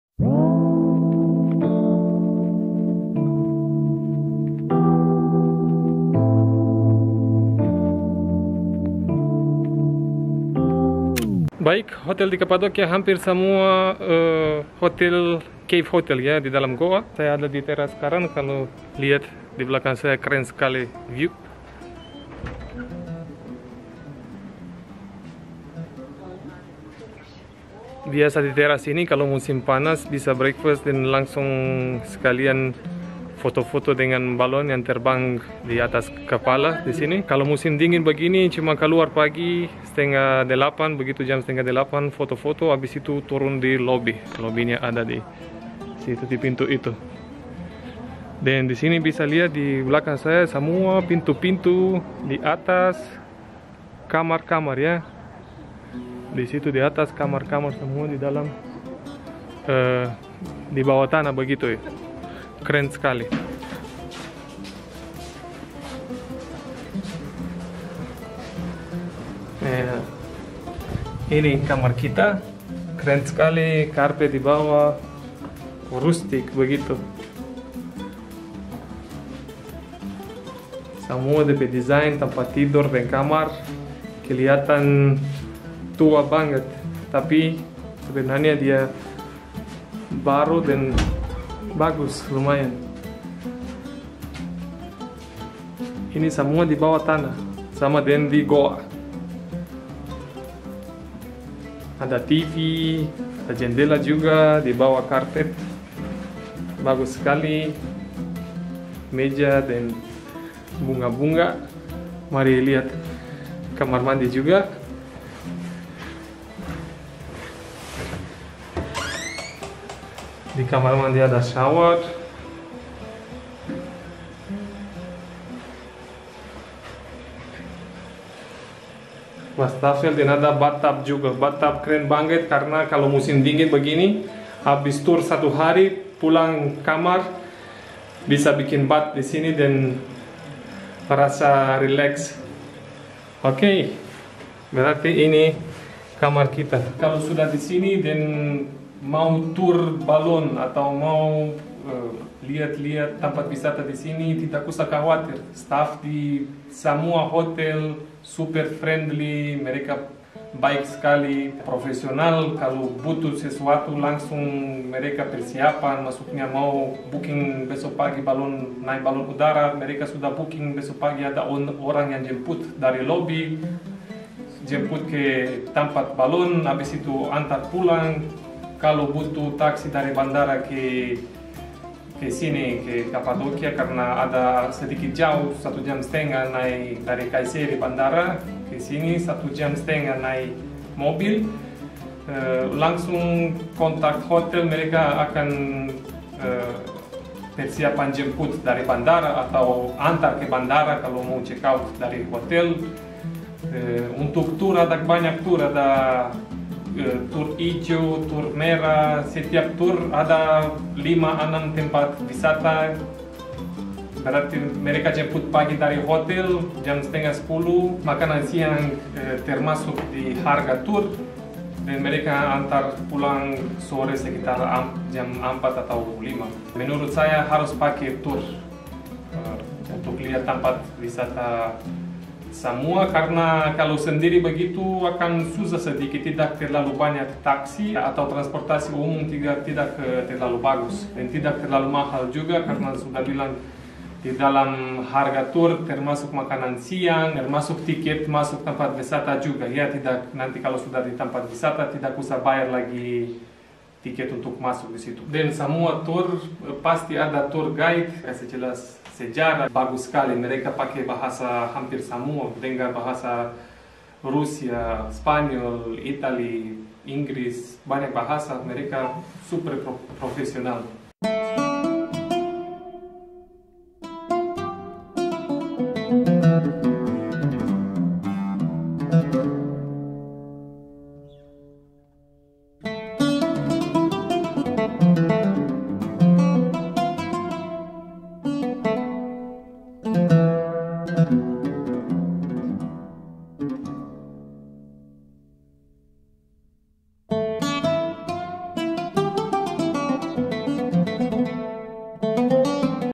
Hotel Dalam Gua!!! | Hotel Unik di Cappadocia | Cave Hotel Room Tour | Elite Cave Hotel
Hampir semua Hotel di cappadocia merupakan cave hotel. Bisa di lihat dari room tour hotel tempat kami menginap, sangat cantik, unik dan yang pasti punya fasilitas yang lengkap layaknya hotel hotel normal biasanya.